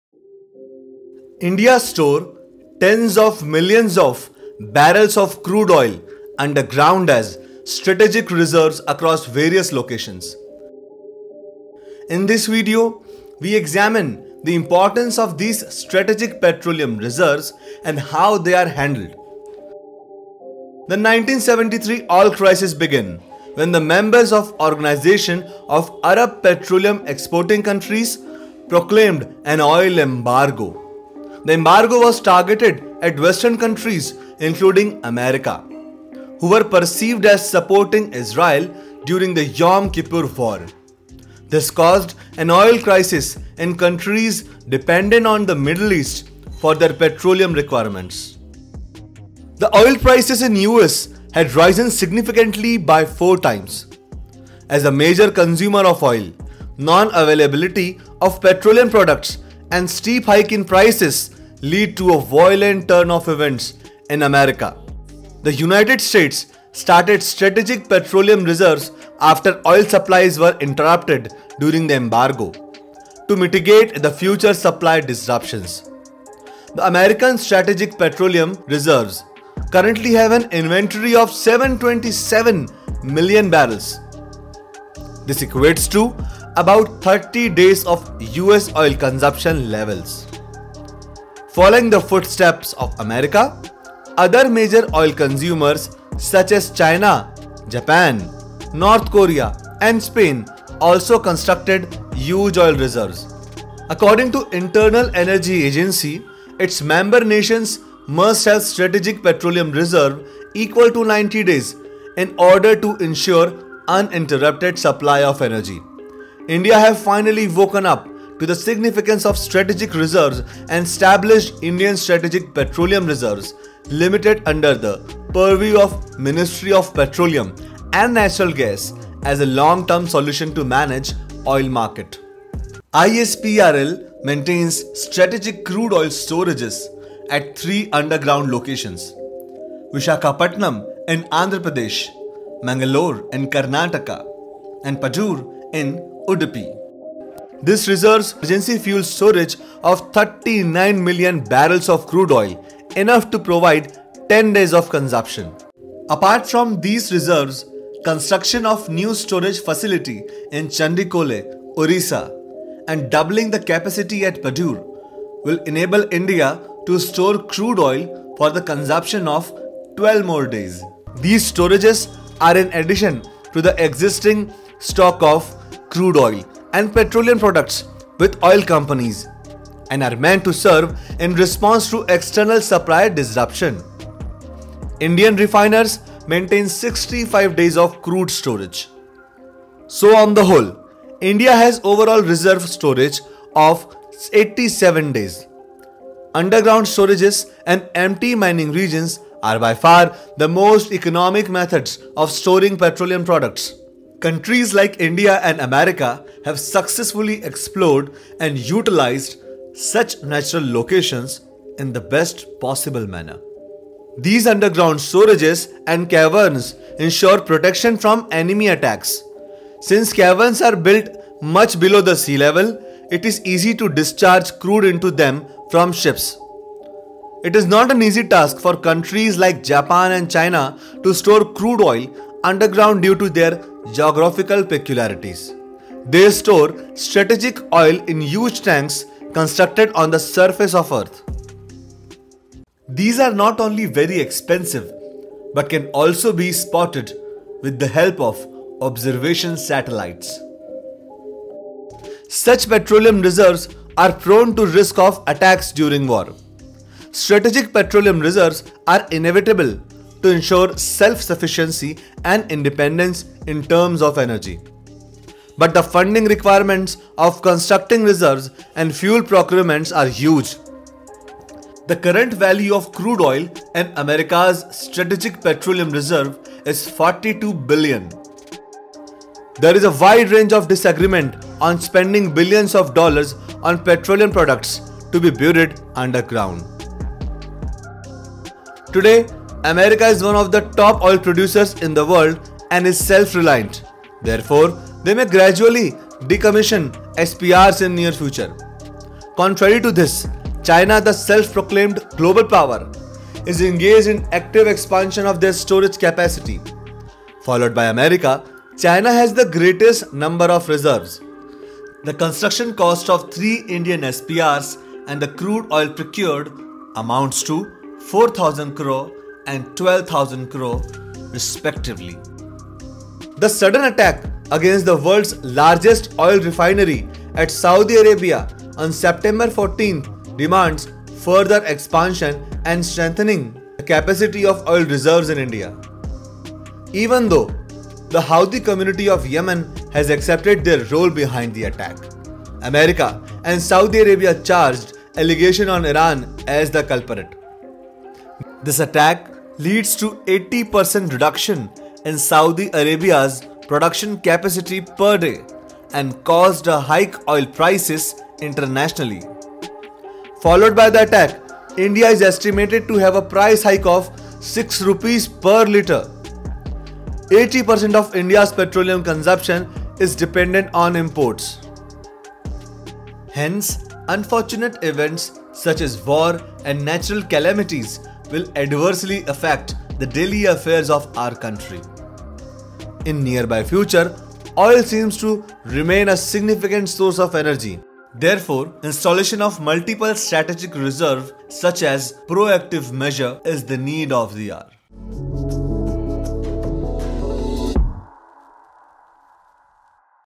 India's Strategic Petroleum Reserve
India is storing tens of millions of barrels of crude oil underground as strategic reserves across various locations. In this video, we examine the importance of these strategic petroleum reserves and how they are handled.